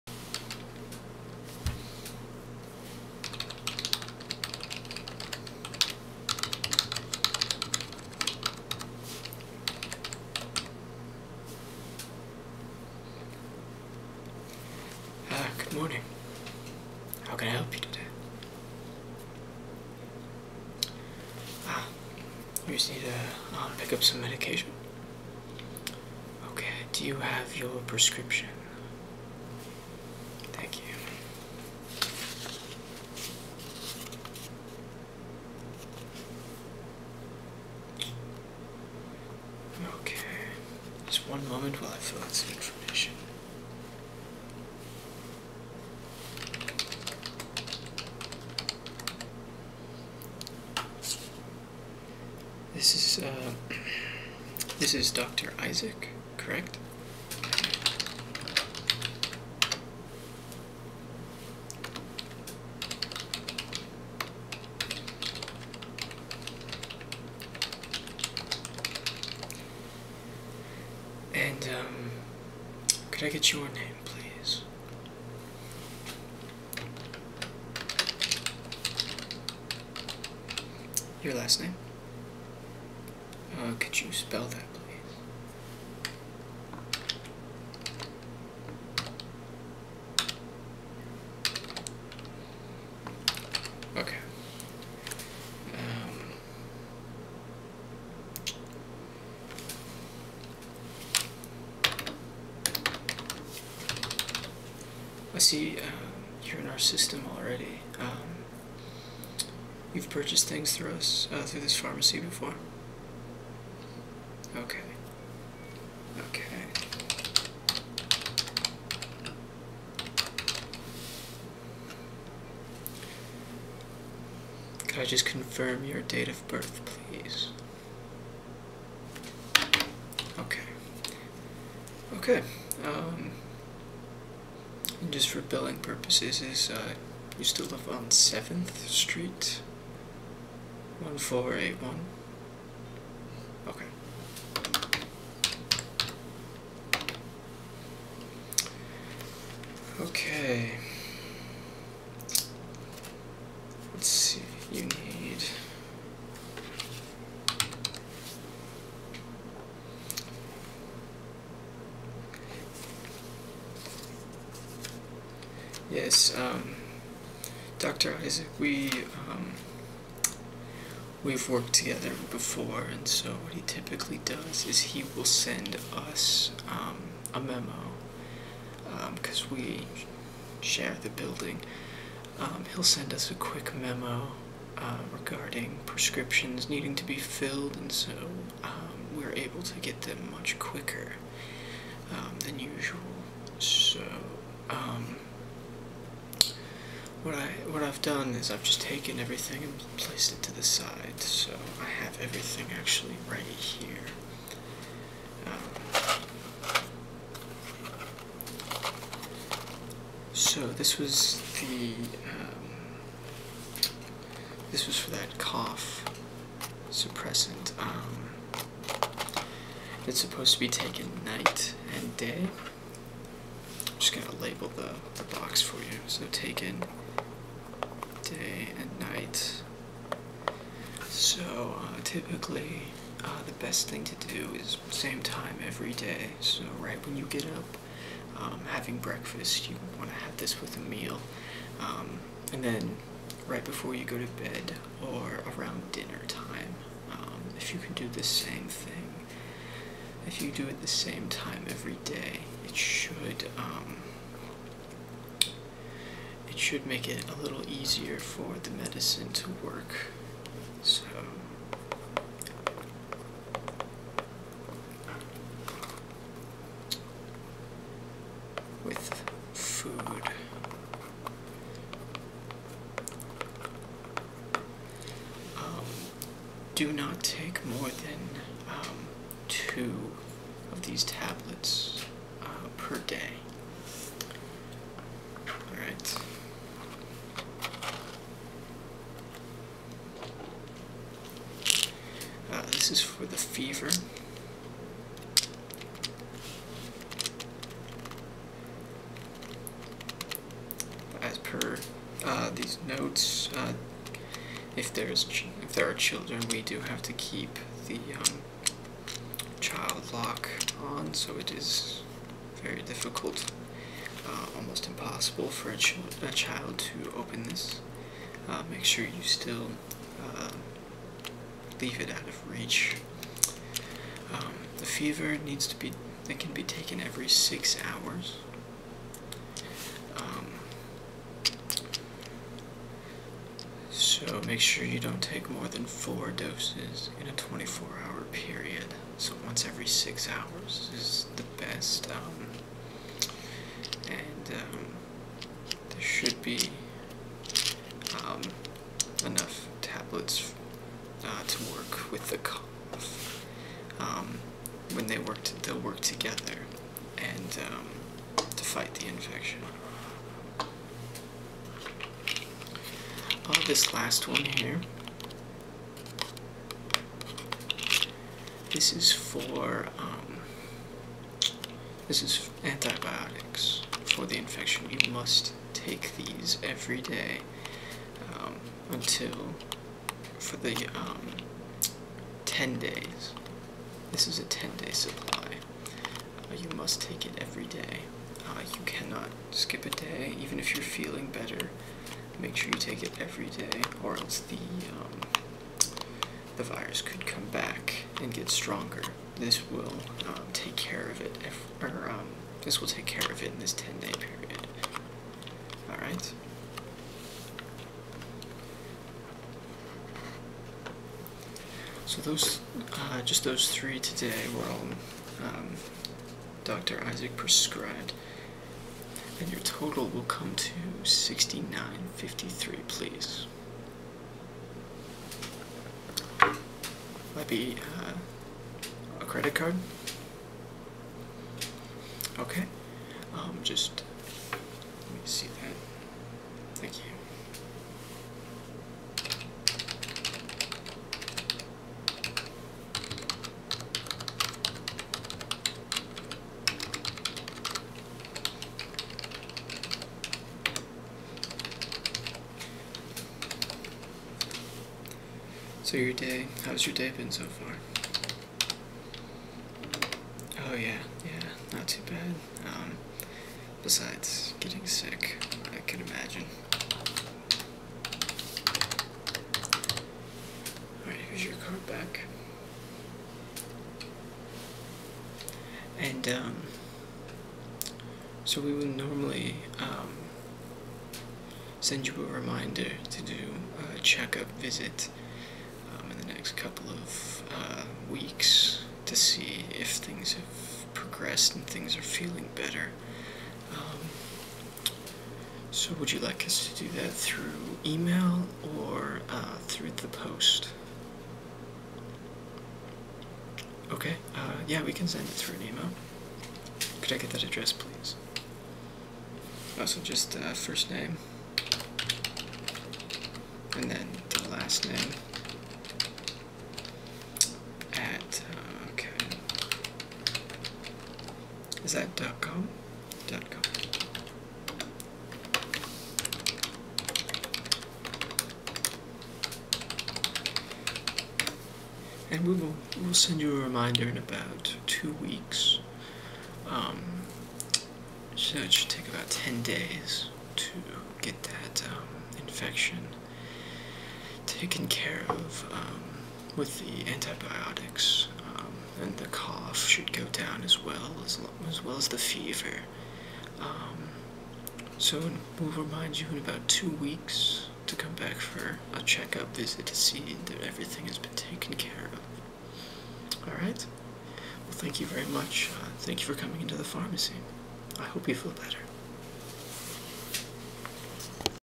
ASMR - Pharmacist (Role Play)
Sit back and relax as I pretend to be your Pharmacist who is giving you the medication that Doctor Issac gave you in the Doctor Check Up video.

I am not a Doctor nor am I a Pharmacist. This is all for pretend make believe and relaxation purposes.